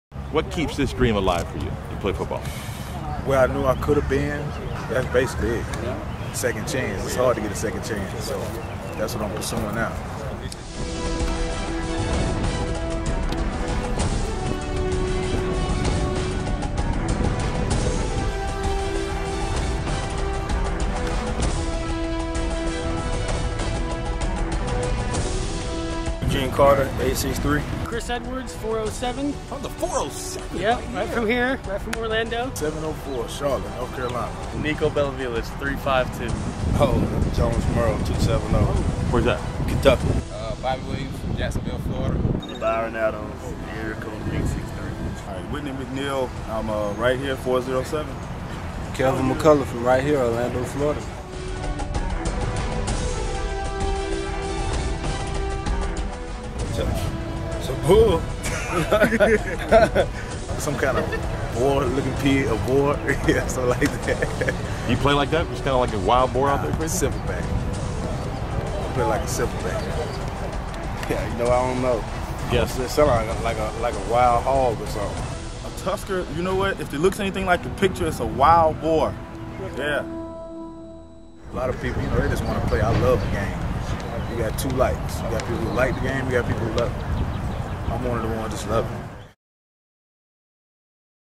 Florida Tuskers Tryout, Citrus Bowl, Orlando, FL, May 15, 2010
Head Coach Jay Gruden and the Florida Tuskers held their first tryout for the 2010 UFL season on May 15, 2010 at the Florida Citrus Bowl